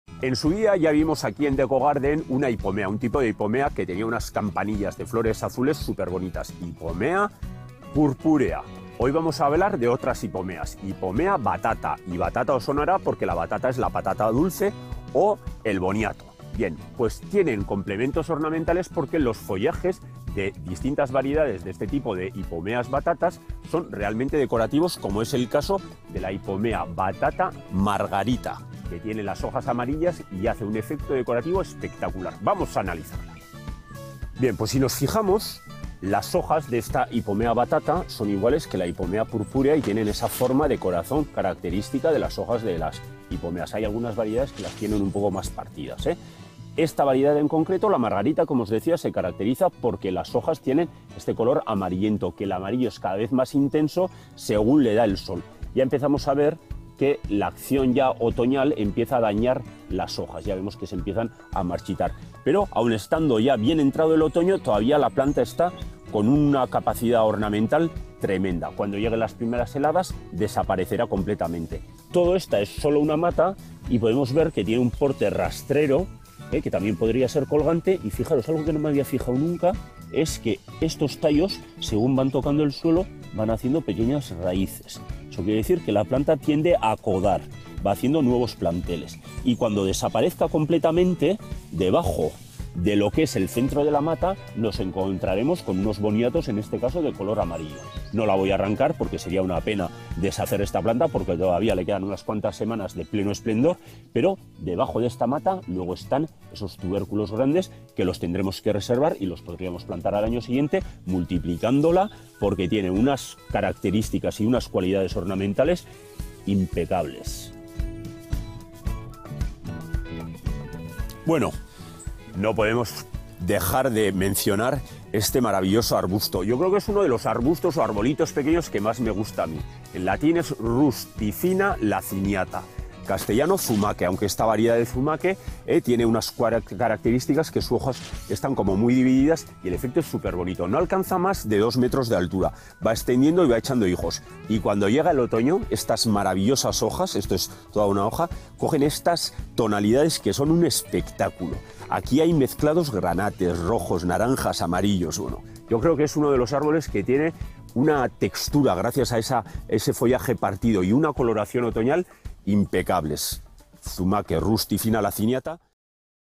Ipomoea batata “margarita” en el jardín - Decogarden - Jardinatis
En esta ocasión, vamos a analizar la zona de un jardín en la que podemos encontrar la Ipomoea batata como planta protagonista.

Esta planta se caracteriza por tener las hojas amarillas y hacer un efecto decorativo espectacular.

Si nos fijamos en las hojas, podemos observar que tienen forma de corazón como el resto de las variedades de Ipomoea.

¿Quieres más información?

🌼 ¡Comparte tu pasión por la jardinería! 🌼

- JARDINATIS -